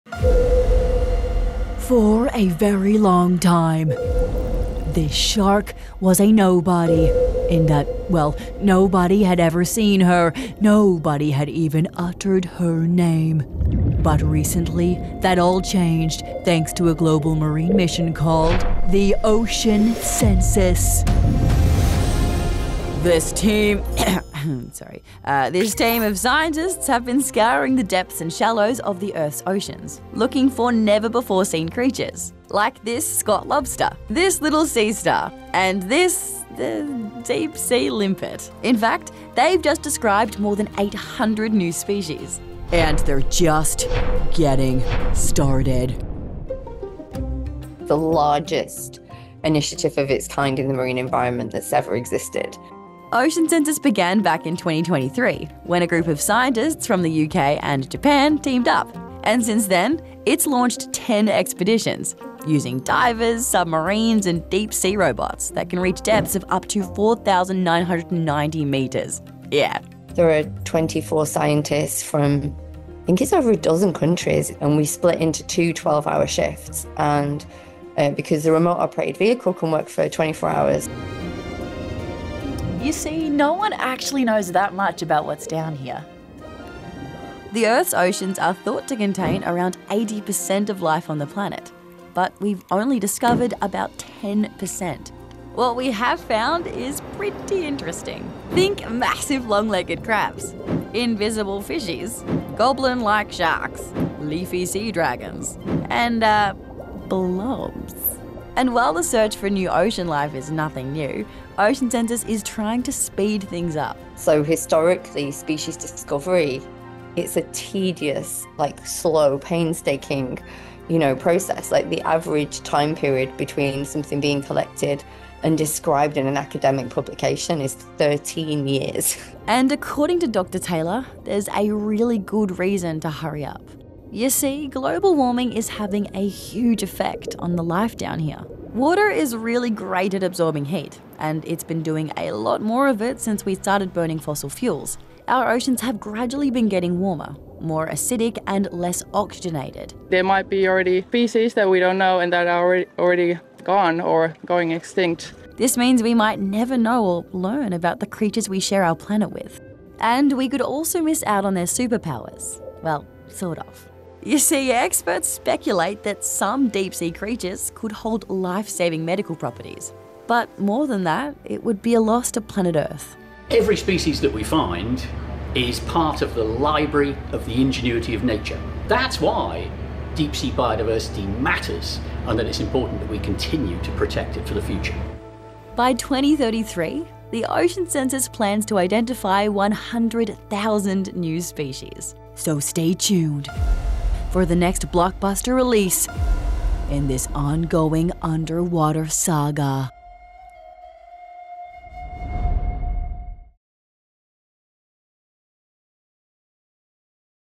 Ocean Census - Behind the News
The world's oceans are full of strange and fascinating creatures, many of which have never been seen by humans. But a project called Ocean Census is setting out to find as many of them as possible. It's just identified 800 new species, and it's just getting started.